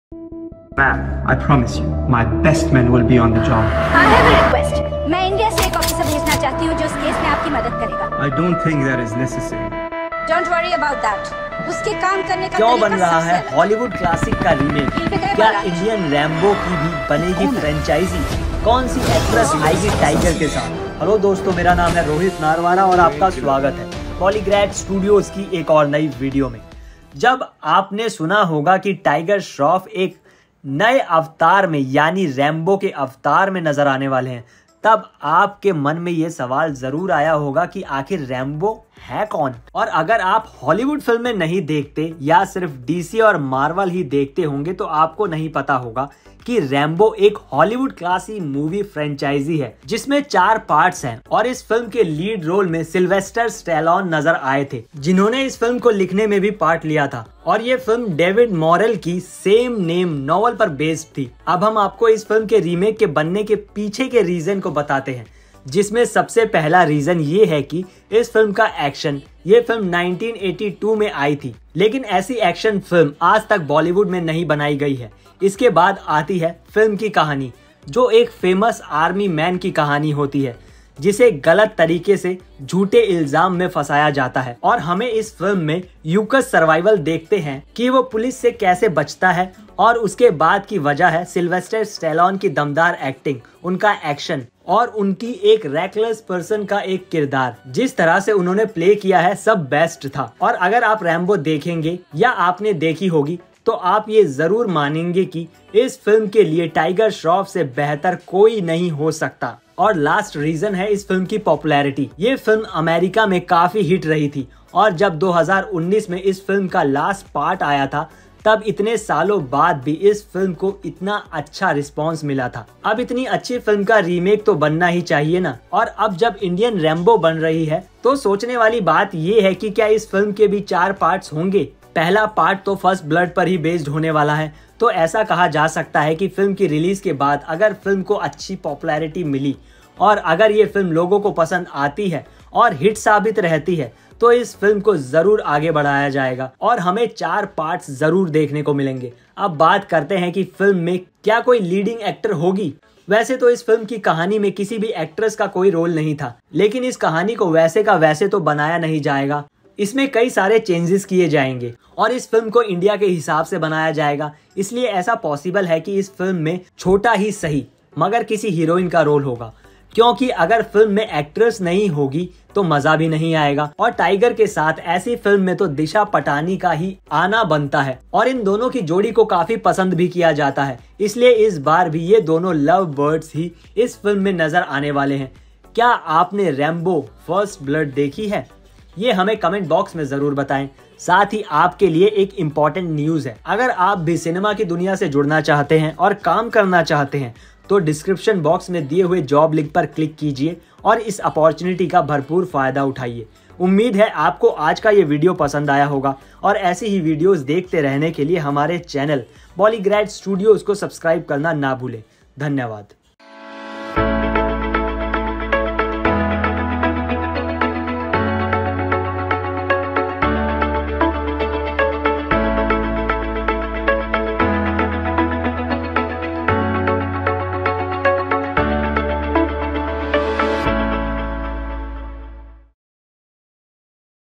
Rambo | 24 Interesting Facts | Tiger Shroff | Kriti Sanon | Vidyut Jamwal | Siddharth Anand | ACTION
Here are some Interetsing and amazings Facts  about the movie Rambo Starring  Tiger Shroff , Kriti Sanon , Vidyut Jammwal Directed by Siddharth Anand Created by Bollygrad studioz

Recent News :- Disha Patani steals the limelight with her recent post, Tiger Shroff's sister comments, 'Damn, woman'

00:00 Intro
01:00 Rambo film banne Ka kya reason hai ?
02:15 Kou Bhadaya jayega Is film ko aage ?

Tiger Shroff's recent release Heropanti 2, which featured him alongside Tara Sutaria and Nawazuddin Siddiqui, opened to mixed responses from the audiences and critics alike. However, the actor, who has a jam-packed year ahead of him, is super excited about his next project Rambo. While Tiger has an interesting lineup of films in store, which includes Vikas Bahl’s Ganapath and Ali Abbas Zafar's directorial venture Bade Miyan Chote Miyan, he is extremely thrilled and is looking forward to Rambo with Siddharth Anand and Rohit Dhawan.

Speaking exclusively with IndiaToday.in, Tiger Shroff said, "I am very much looking forward to Rambo. It has been in the making for a very long time. I am also looking forward to working with Rohit Dhawan and Siddharth Anand. With Sid, I have an equation through War, so looking forward to working with that team again."

Rambo is the Hindi adaptation of the 1982 Hollywood classic of the same name which featured Sylvester Stallone. Tiger will be stepping into the shoes of the veteran action star. When asked what can we expect from the film, the actor said, "Though it's too early to talk about it right now, one thing is for sure. It is going to be very different from everything that I have done before. I know it's very hard to believe but you will see something soon enough and know why I am saying this."

E - Dhroov Sharma
A - Rohit Narwara
W - Manisha vidhani
Uploaded by :- Palak Jain .